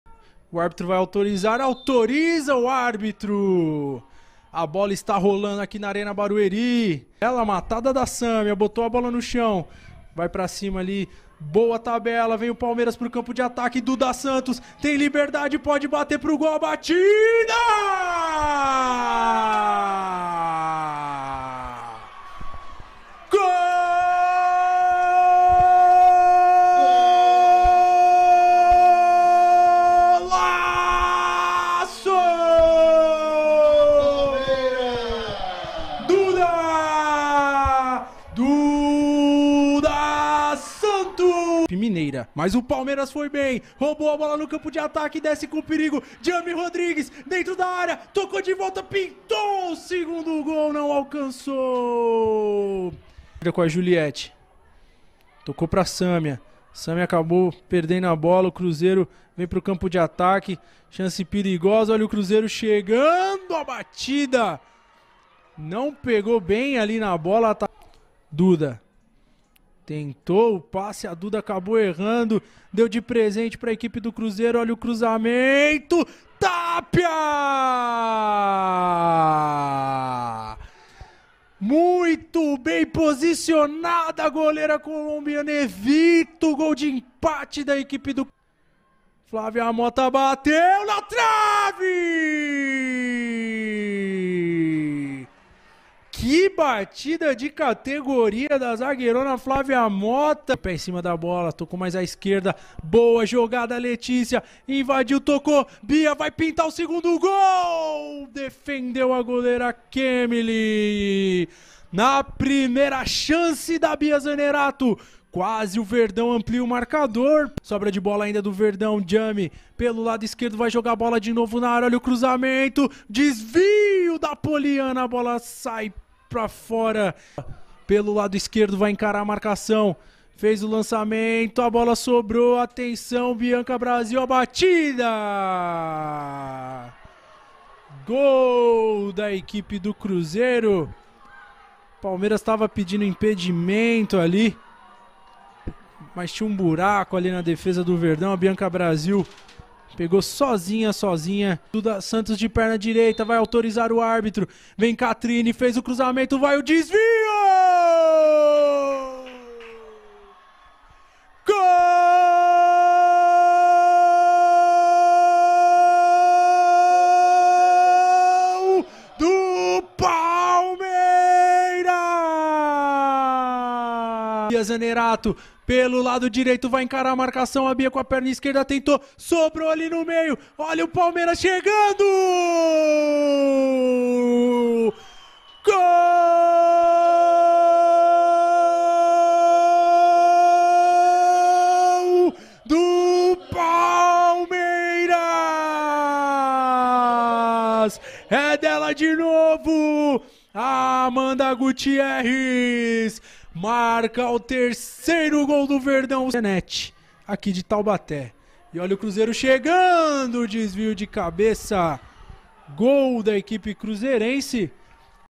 Palmeiras 3 x 2 Cruzeiro - Melhores Momentos Brasileirão Feminino 2023
Palmeiras supera o Cruzeiro e vence a segunda seguida pelo Brasileiro Feminino
Amanda Gutierres sai do banco e faz dois gols para definir o duelo na Arena Barueri.
O Palmeiras venceu o Cruzeiro por 3 a 2 na noite deste domingo, em Barueri, pela 11ª rodada do Brasileiro Feminino. Amanda Gutierres foi o grande destaque do duelo. Ela saiu do banco com o jogo empatado em 1 a 1 e fez dois gols. Antes, Duda Santos tinha marcado pelas Palestrinas no primeiro tempo. Byanca Brasil e Carol Baiana - ambas com passagem pelo Palmeiras - descontaram para as mineiras.

Palmeiras 3 x 2 Cruzeiro feminino
Palmeiras  x  Cruzeiro feminino